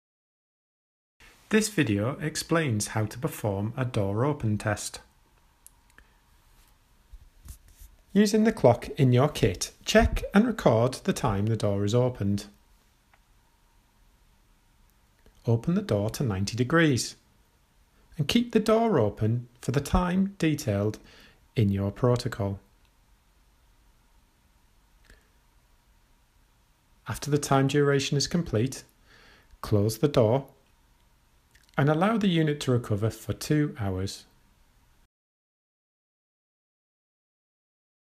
Temperature Mapping - How to perform a Door Open Test
How to perform a door open test using the DIY Temperature Mapping kit.